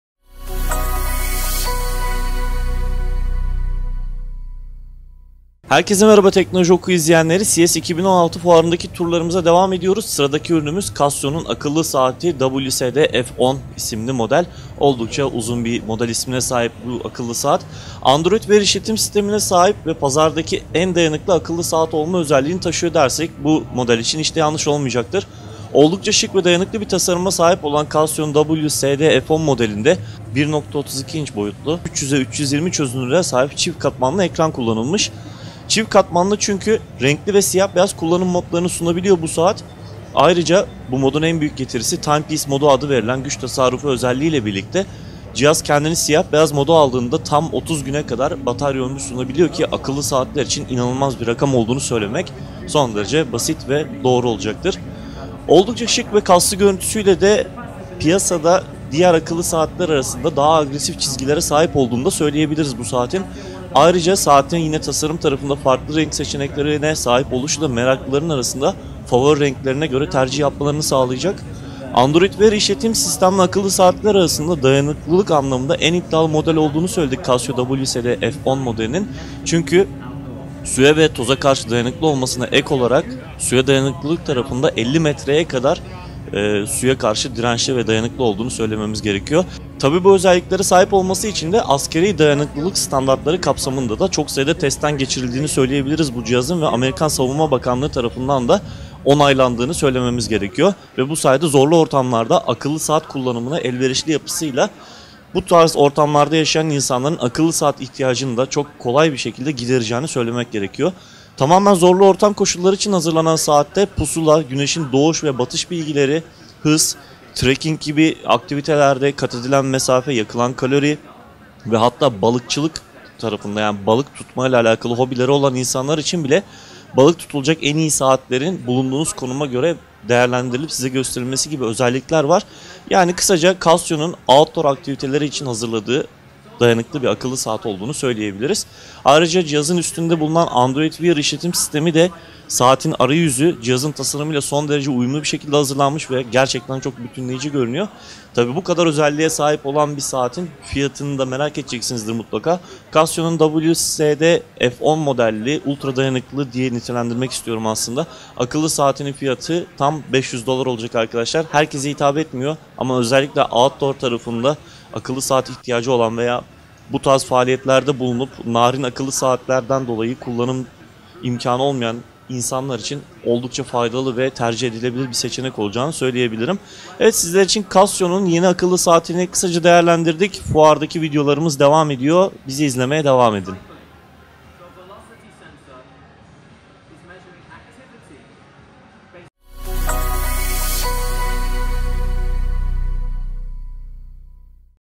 Casio WSD F10 İnceleme CES 2016
Teknolojioku.com olarak yerinde takip ettiğimiz, CES 2016 fuarındaki turumuza devam ediyoruz. Sıradaki ürünümüz olan Casio’nun akıllı saati WSD-F10, Android wear işletim sistemine sahip  pazardaki en dayanıklı akıllı saat olma özelliğini taşıyor.

Oldukça şık ve dayanıklı bir tasarıma sahip olan Casio WSD-F10 modelinde 1.32 inç boyutlu 300 x 320 çözünürlüğe sahip çift katmanlı ekran yer alıyor. Renkli ve siyah beyaz kullanım modu kullanma imkanı sunan Casio, WSD-F10 modeli ile Timepiece Mode adını verdiği güç tasarrufu modu ile 30 güne kadar pil ömrü vaadediyor.

Bu saat şık ve kaslı görüntüsüyle de piyasadaki diğer akıllı saatlere oranla daha agresif çizgilere sahip.  Ayrıca saatin farklı renk seçenekleri olusu da meraklıları arasında favrori renklerine  tercih yapmalarını sağlayacak

Android Wear işletim sistemli akıllı saatler arasında, dayanıklılık anlamında en iddialı model olan Casio WSD-F10 modeli, suya ve toza karşı dayanıklı olarak geliştirildi ve 50 metreye kadar suya dayanıklı olduğu açıklandı.

Askeri dayanıklılık standartları kapsamında da bu özellikleri onaylanan saat, zorlu ortamlarda akıllı saat kullanımına elverişli yapısıyla farklı bir sınıfta yer alacak.

Tamamen zorlu ortam koşulları için hazırlanan saat, pusula, güneşin doğuş ve batış bilgileri, hız, katededilen mesafe, gibi outdoor aktivitelerin takibine de imkanı sunabiliyor.

Android Wear işletim sistemi ile gelen saatin arayüzü, cihazın tasarımıyla son derece uyumlu görünüyor.
Casio’nun akıllı saatinin fiyatı ise 500$ olacak.

Bu içerikleri de izleyebilirsiniz:

İncelemeler

Röportajlar

Nasıl yapılır?

Kutudan çıkıyor

Otomobil

Oyun inceleme